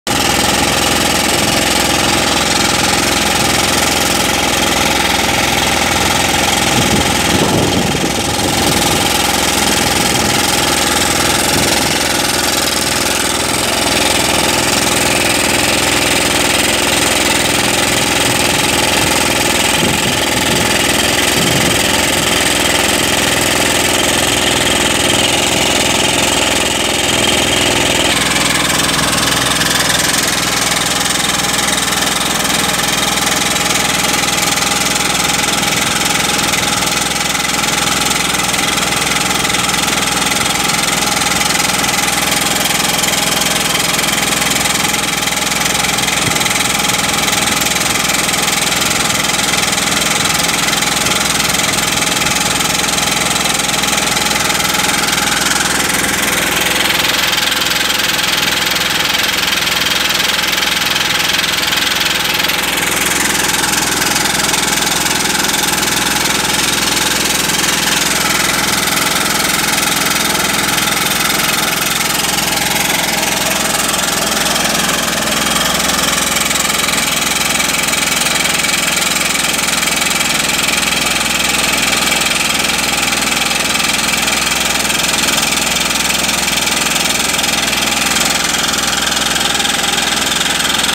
Mindrill - Pneumatic Crawler Drill Onsite Video
MINDRILL CD100 crawler drill in action

1. MINDRILL CD100 DTH / CD115 TH Crawler Drill is a versatile choice for quarries, open-pit mining, and construction needs

2. It can be used with MD100 DTH hammer or MD690 pneumatic drifter/top hammer with a quick interchangeability options

3. It is a single-man operated, high productivity drilling rig with great return on investment

for all your mining & construction needs!